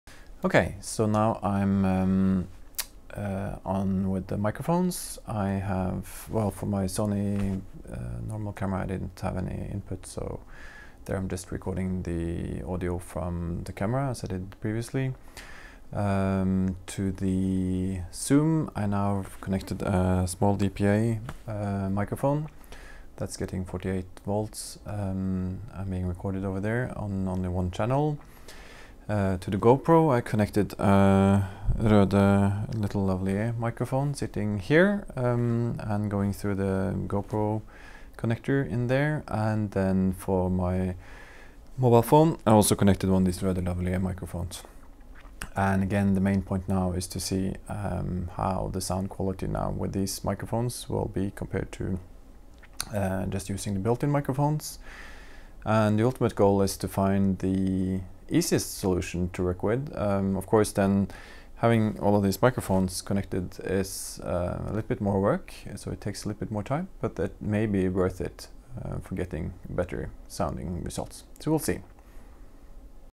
Audio/video test of Zoom Q8 with DPA 4060
Testing how the Zoom Q8 and DPA 4060 works for interviews.